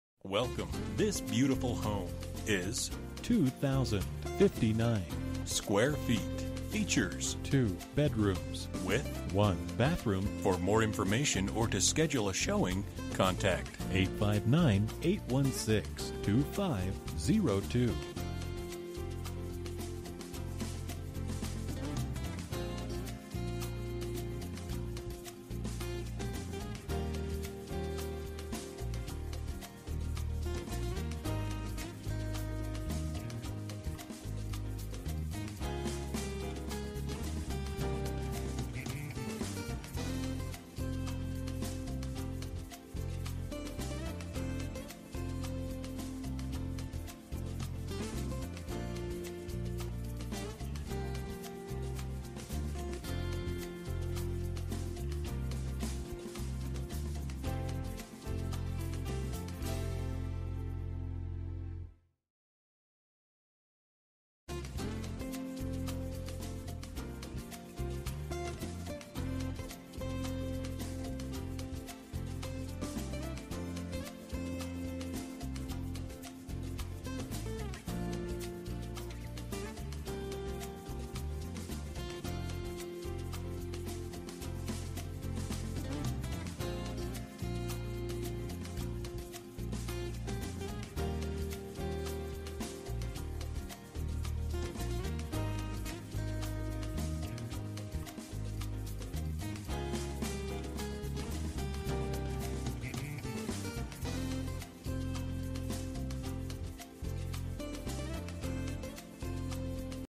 Residential for sale - 227 E 7th Street, Newport, KY 41071
Welcome home to this 2 bedroom home in the heart of Newport! Just minutes to downtown, restaurants, and shopping this is a great place to unpack your bags!  Original hardwood floors throughout, updated bathroom & kitchen and HUGE bedrooms with high ceilings! This home has been completely brought back to life. New furnace, new plumbing and it is move-in ready!  Hard to find fenced-in backyard is great for entertaining outdoors!  Call today for a showing!

Bedrooms: 2
Bathrooms: 1
Square Feet: 2059
Price: $109,900

For more information about this property, please contact Katie Robbins Realty at or . You can also text 5737339 to 67299.

10/02/2019 03:15:19 am

Last modified: